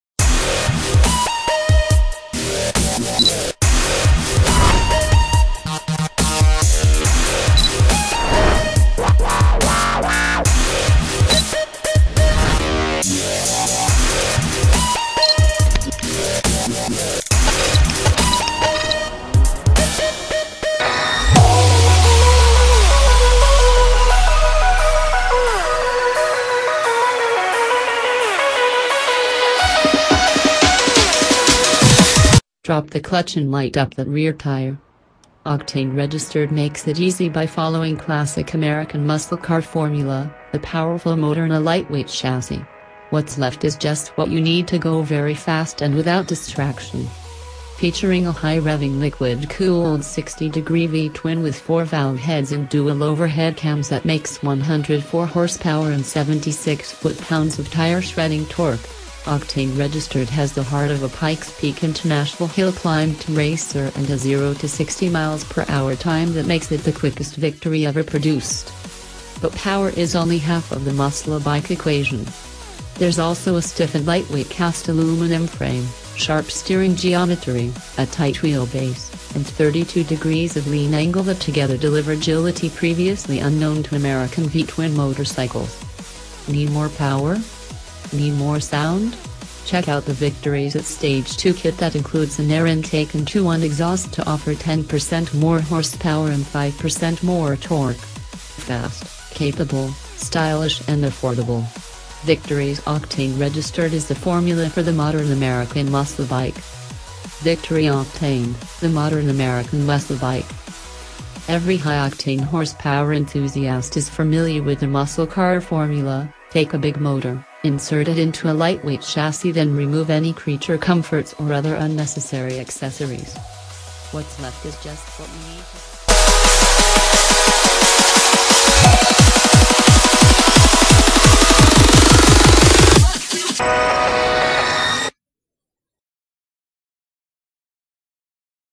WOW AMAZING Low Price !!! 2017 Victory Octane Price & Spec
WOW AMAZING Low Price !!! 2017 Victory Octane Price & Spec -Drop the clutch and light up that rear tire. Octane® makes it easy by following classic American musclecar formula: a powerful motor in a lightweight chassis. What’s left is just what you need to go very fast and without distraction. Featuring a high revving liquid-cooled 60-degree V-Twin with four-valve heads and dual-overhead cams that makes 104 horsepower and 76 foot-pounds of tire-shredding torque, Octane® has the heart of a Pikes Peak International Hill Climb™ racer and a 0-60 mph time that makes it the quickest Victory ever produced. But power is only half of the musclebike equation. There’s also a stiff and lightweight cast-aluminum frame, sharp steering geometry, a tight wheelbase, and 32 degrees of lean angle that together deliver agility previously unknown to American V-Twin motorcycles. Need more power? Need more sound? Check out the Victory’s Stage 2 Kit that includes an air intake and 2-1 exhaust to offer 10% more horsepower and 5% more torque. Fast, capable, stylish and affordable. Victory’s Octane® is the formula for the Modern American Musclebike.

$10,499